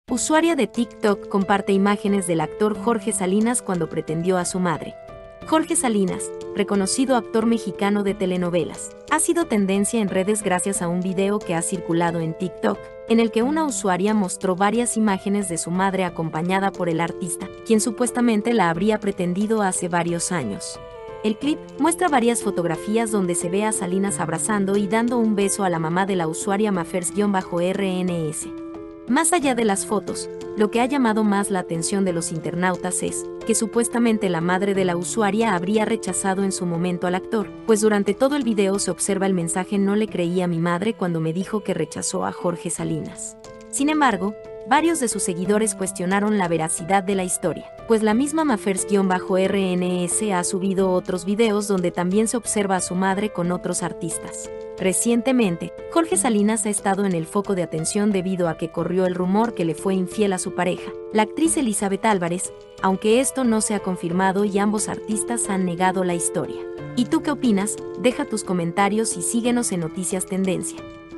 USUARIA COMPARTE IMAGENES DE JORGE SALINAS CUANDO LO RECHAZO DE JOVEN
Jorge Salinas, reconocido actor mexicano de telenovelas, ha sido tendencia en redes gracias a un video que ha circulado en el que una usuaria mostró varias imágenes de su madre acompañada por el artista, quien supuestamente la habría pretendido hace varios años.